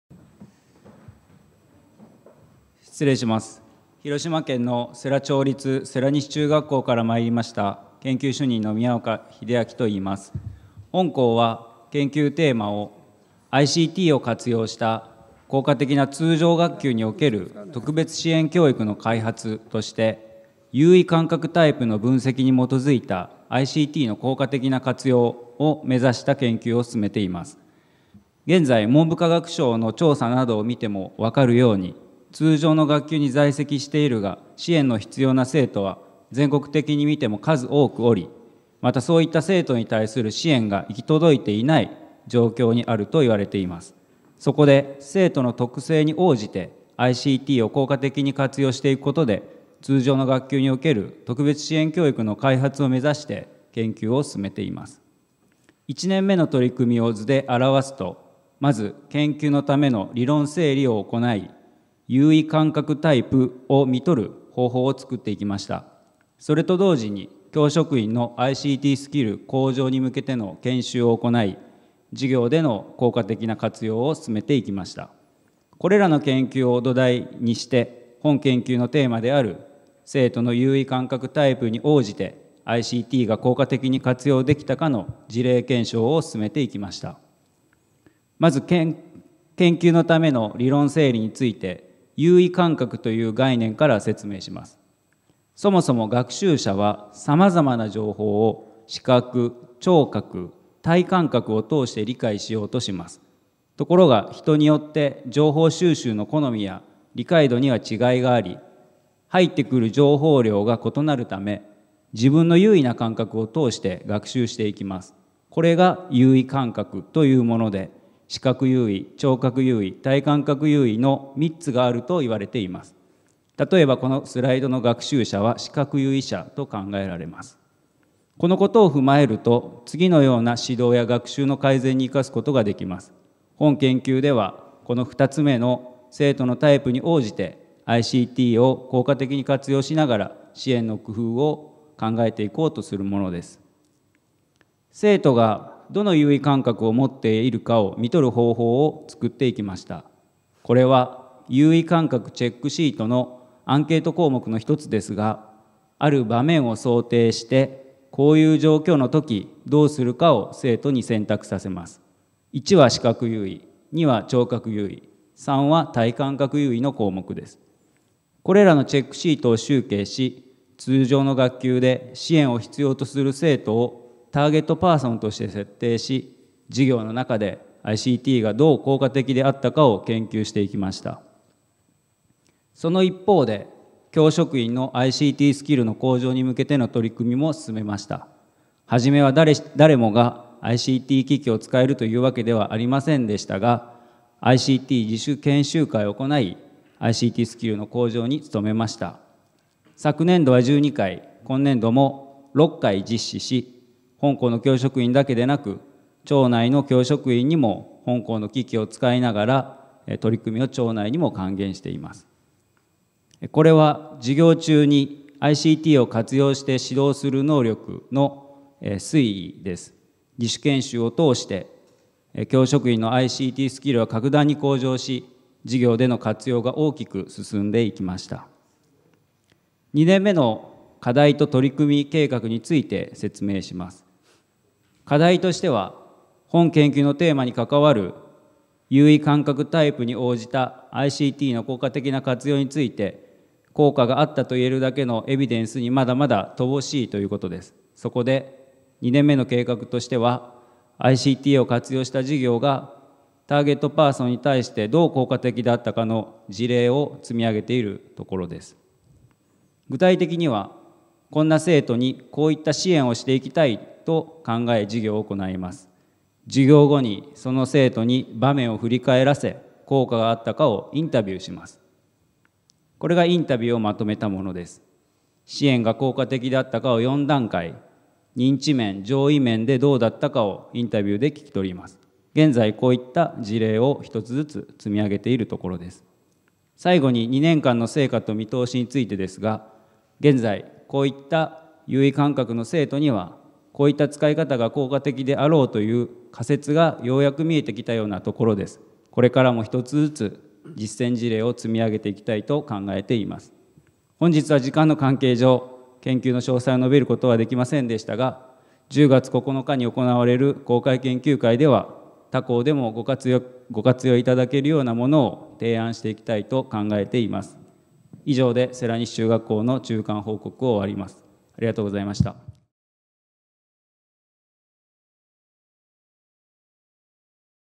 世羅町立世羅西中学校│中間成果報告│平成26年度実践研究助成成果報告会
パナソニック教育財団は、平成26年7月30日（水）、東京において「平成26年度成果報告会」を開催しました。
成果報告会では、特別研究指定校の取り組みを通じて、教育現場での課題解決に向けて、校内研究やツールとしてのICTの活用をどのように定着させていけばよいのかというテーマで話し合いをしました。

この動画では、世羅町立世羅西中学校の中間成果報告をご覧いただけます。